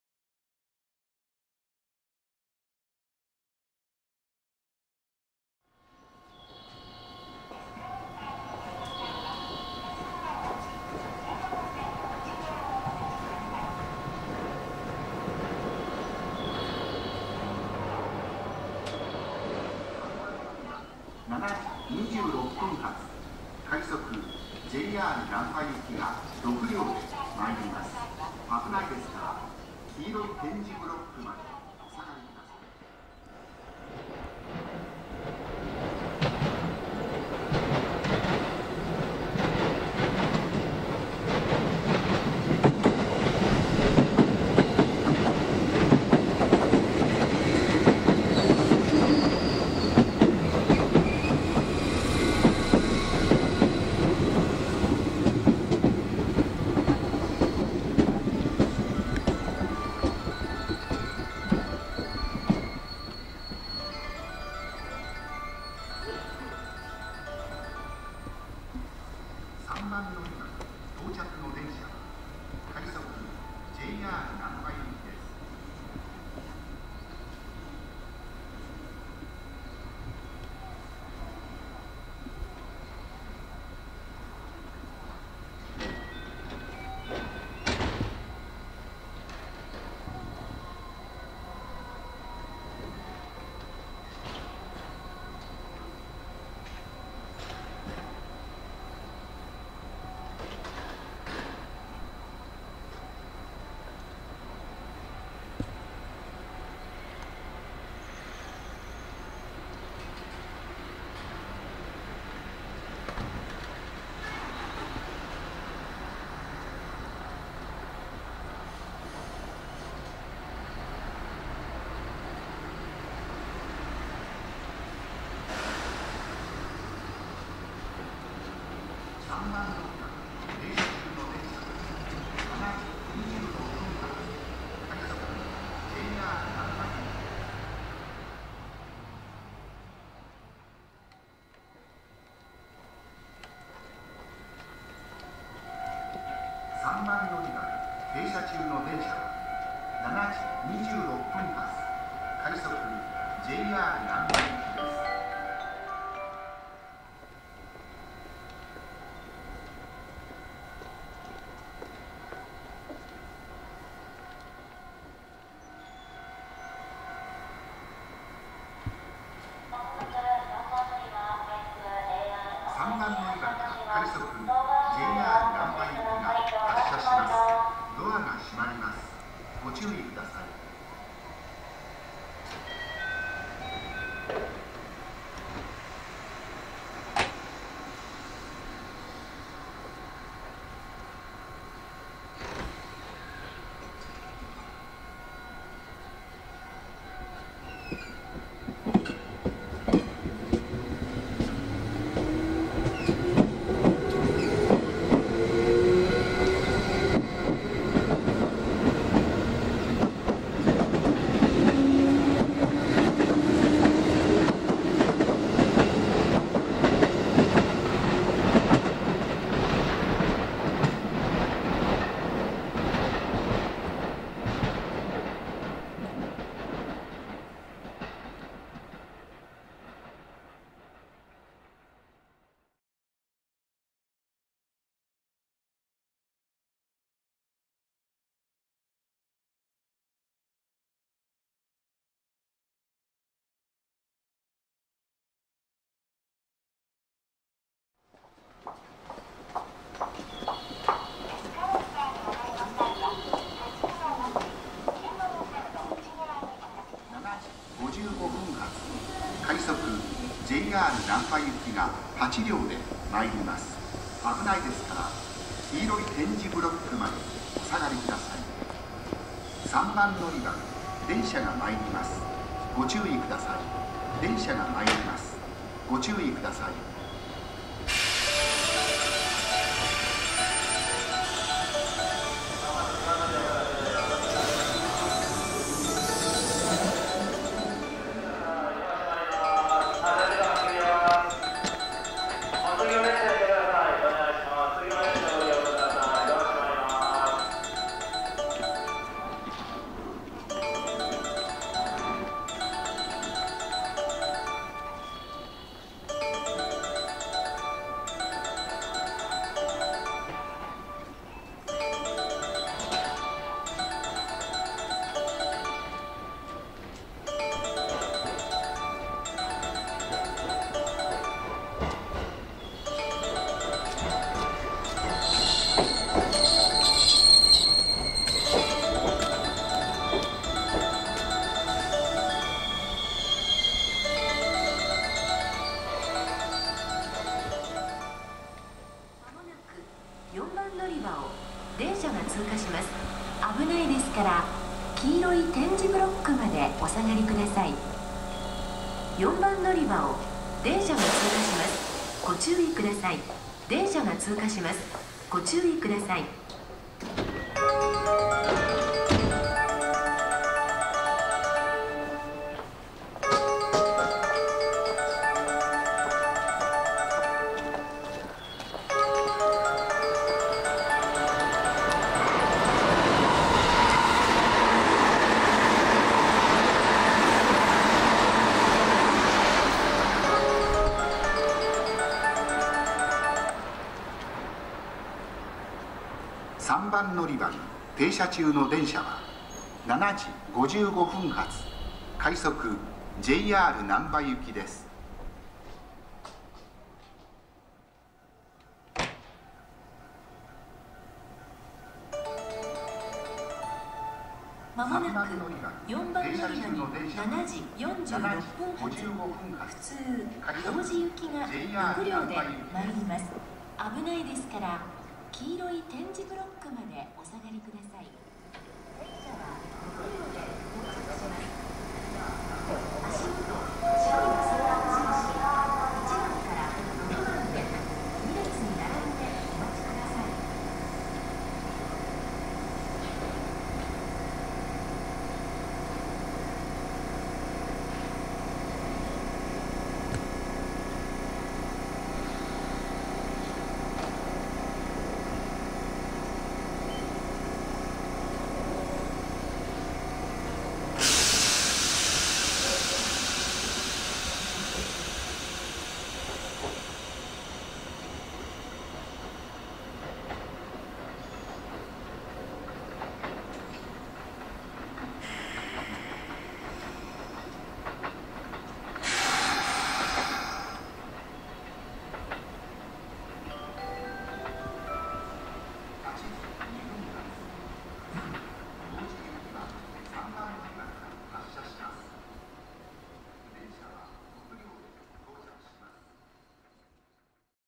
ダイヤ改正で消滅 柏原始発 JR難波行き「柏原快速」
2022年3月のダイヤ改正で消滅してしまう柏原始発 JR難波行きの快速 (通称：柏原快速) を柏原駅で撮影してきました。平日の朝、7時26分発と7時55分発の2本が設定されており、回送列車到着～発車までを撮影しました。